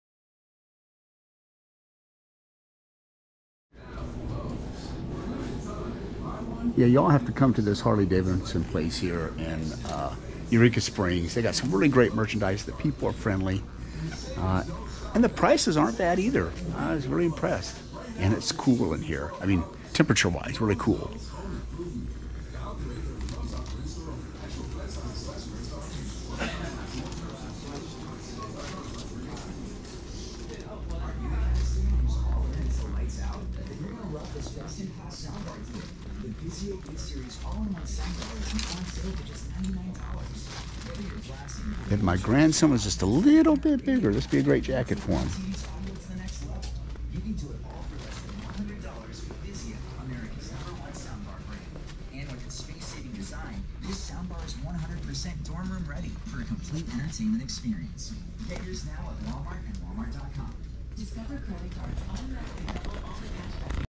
Another America By Motorcycle QUICKIE
An Indian Rider goes into a H-D store!